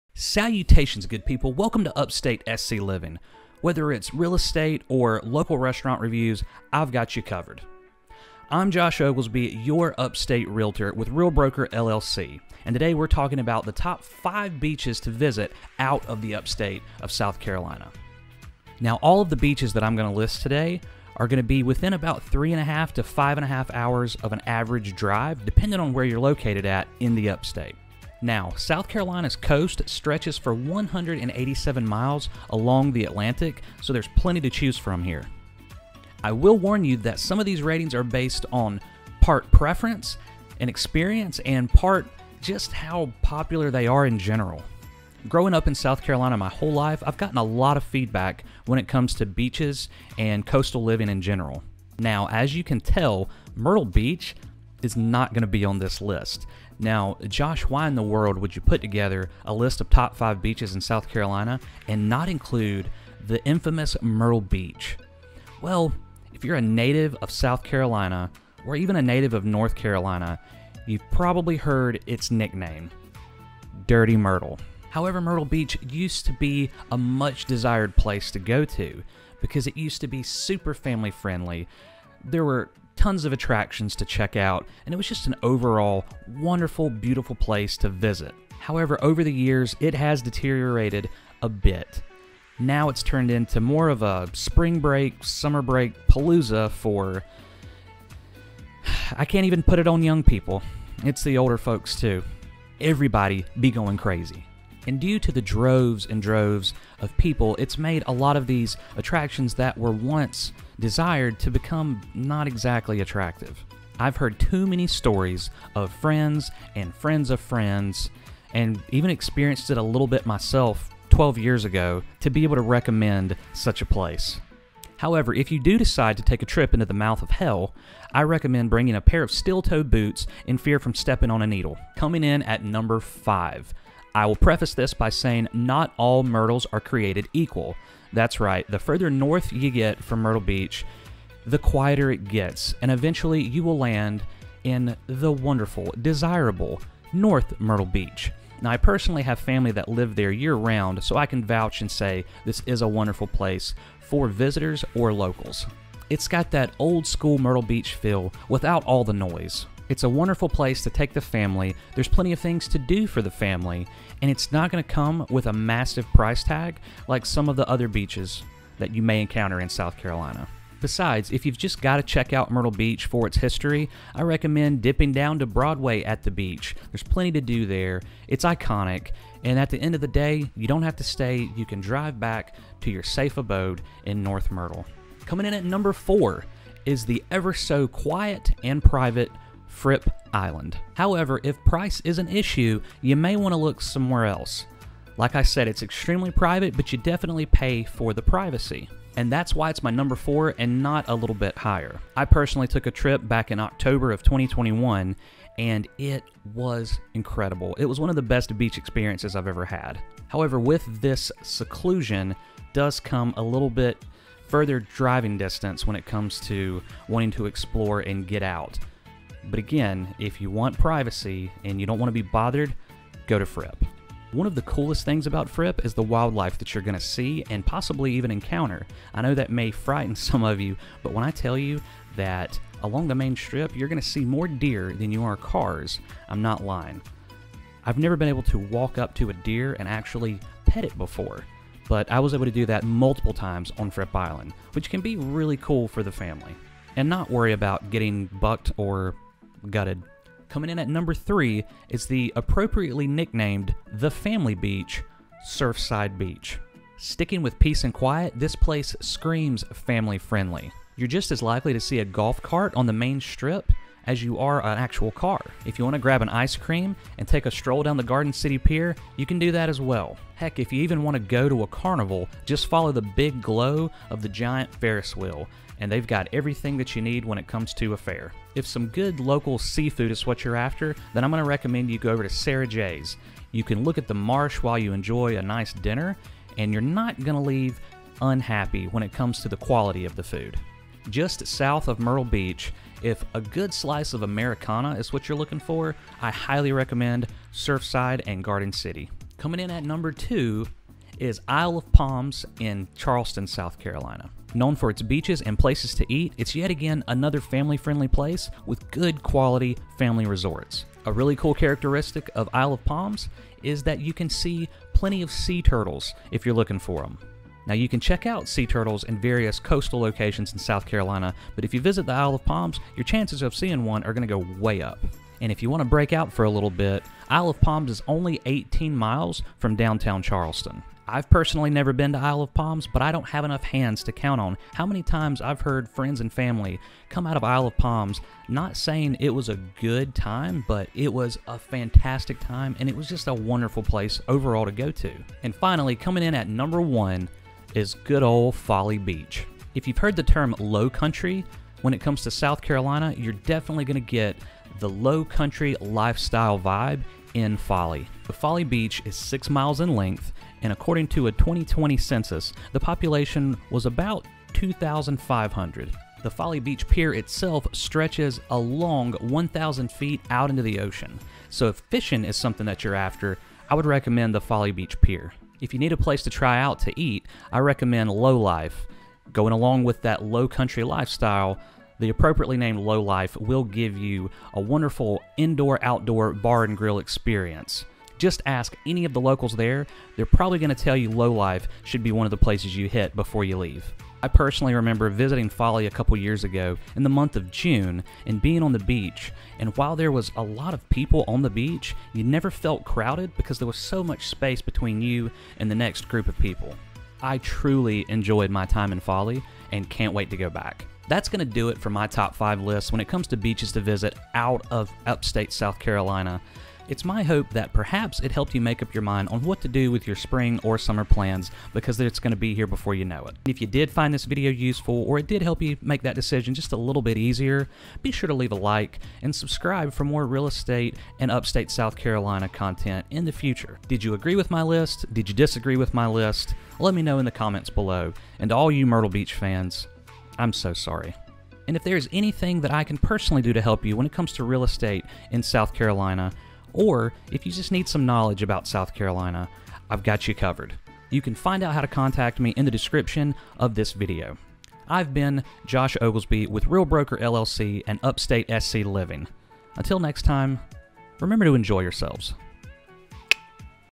Top 5 Beaches Near Upstate SC!
These are the top 5 beaches in my humble South Carolina opinion. With 187 miles of beach, the South Carolina coastline has a lot to offer and choose from! Did I get the top 5 right?

For any and all questions regarding Real Estate in SC, don't hesitate to reach out! Looking to buy or sell? I can help!